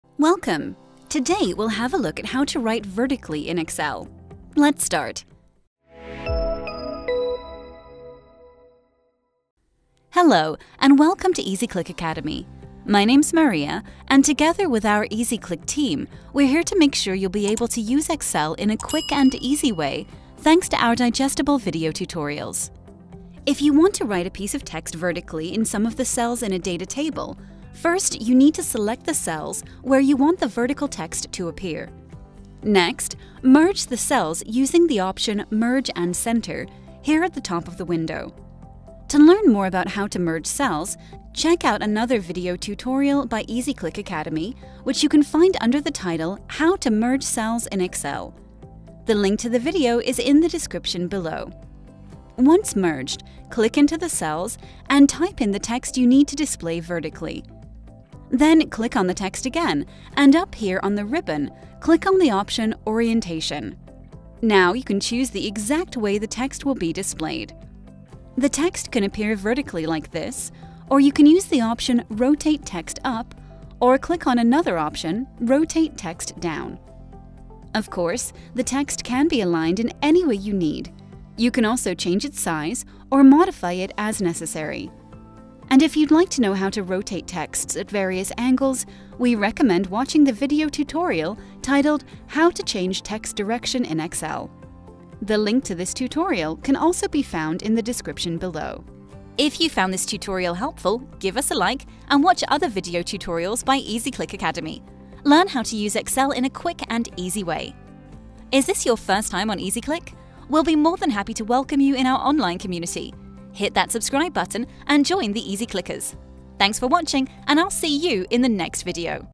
How to Write Vertically in Excel (Quick and Easy)
Today we’ll have a look at how to write vertically in Excel. Let’s start!

⏱️Timestamps⏱️
0:00 Intro
0:29 How to Merge Cells in Excel
0:55 How to Display Text Vertically